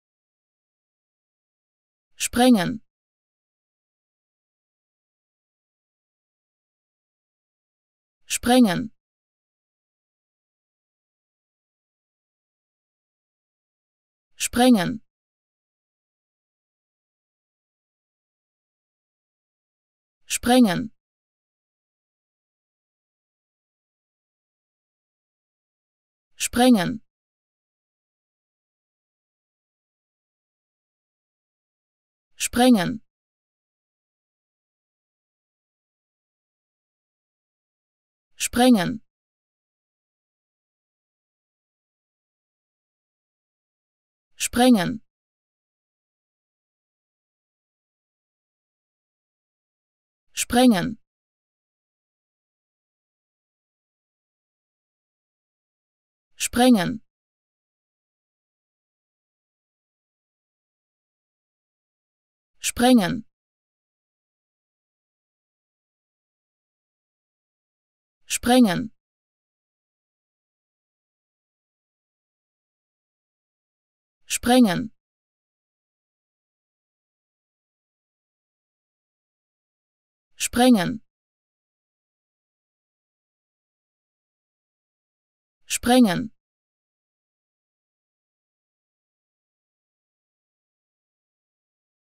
How to Pronounce ''sprengen'' (bust) Correctly in German
Learn how to say ''sprengen'' in German with this short pronunciation tutorial.